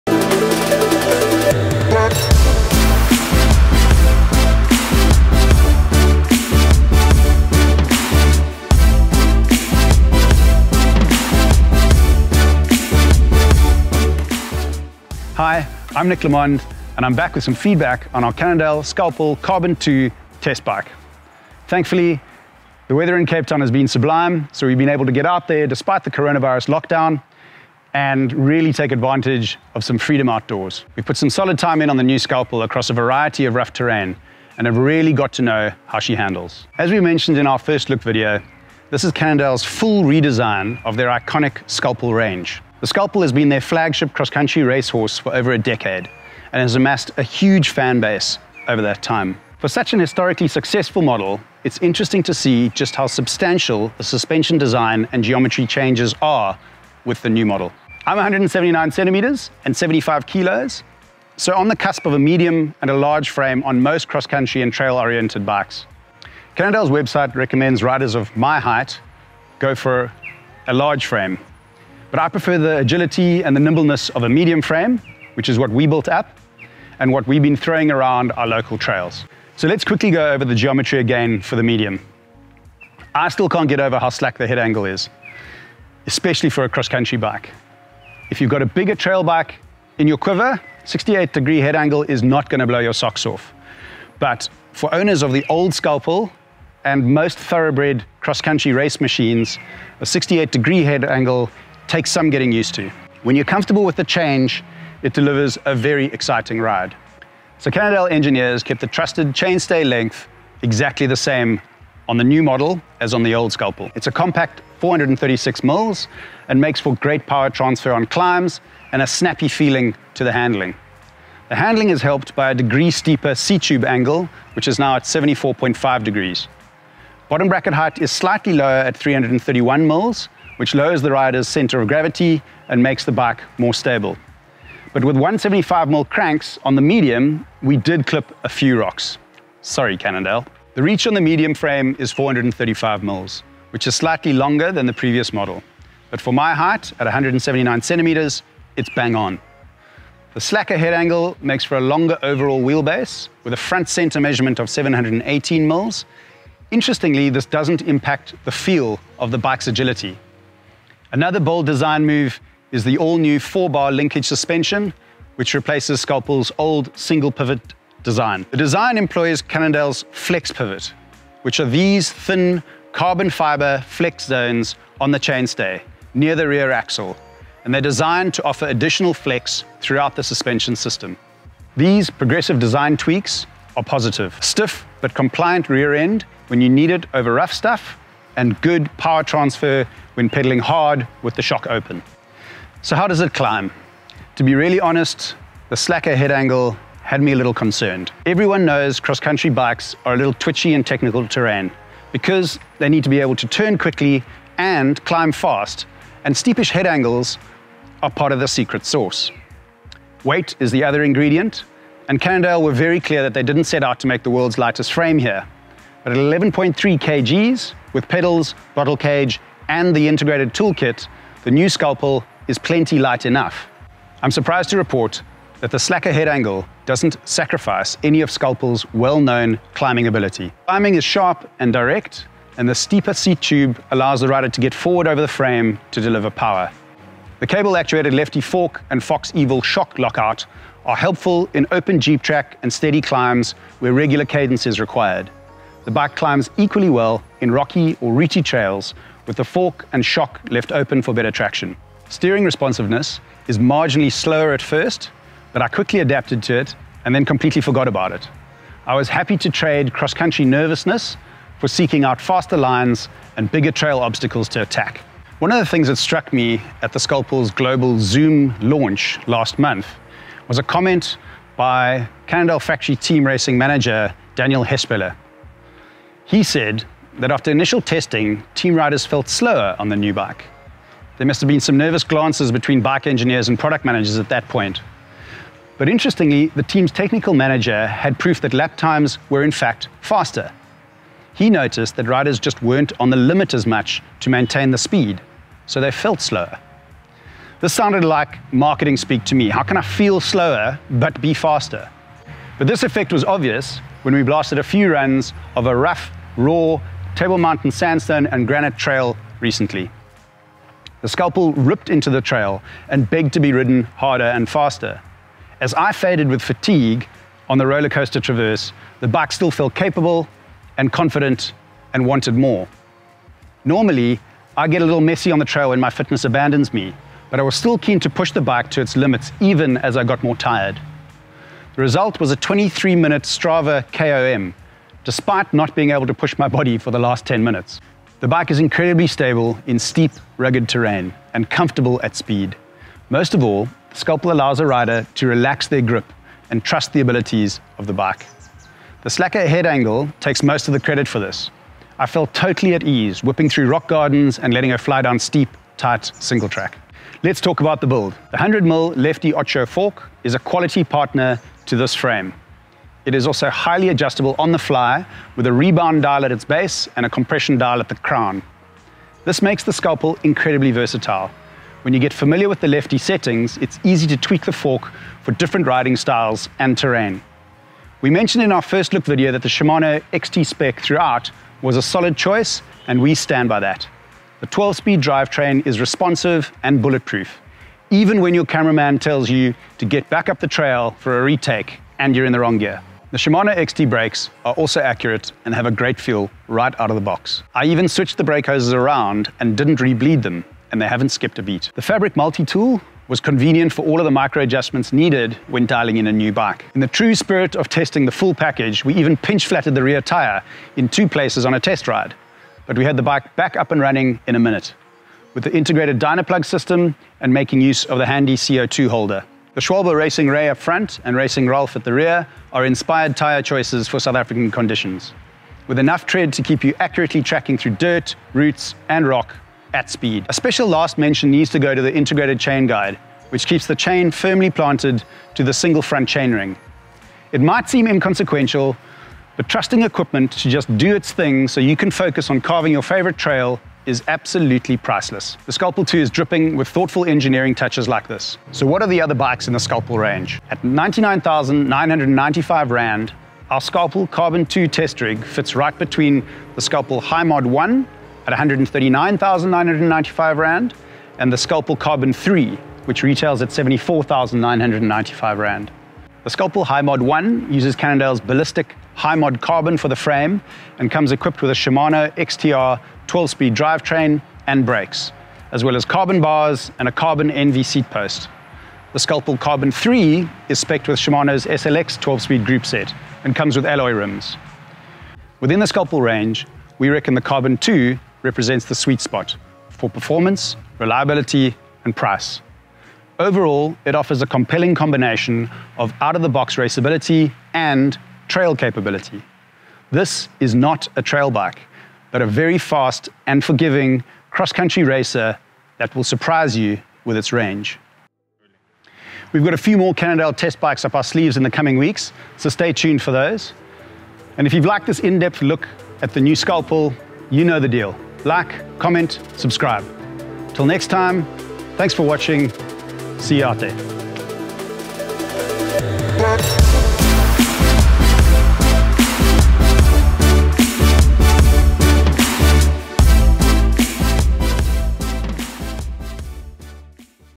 FULL TEST & REVIEW | 2021 CANNONDALE SCALPEL CARBON 2 | 4K
We brought you our “FIRST LOOK” a couple of weeks ago but now it’s time to buckle up for our in-depth review of how the 2021 Cannondale Scalpel Carbon 2 rides and handles South African conditions🤟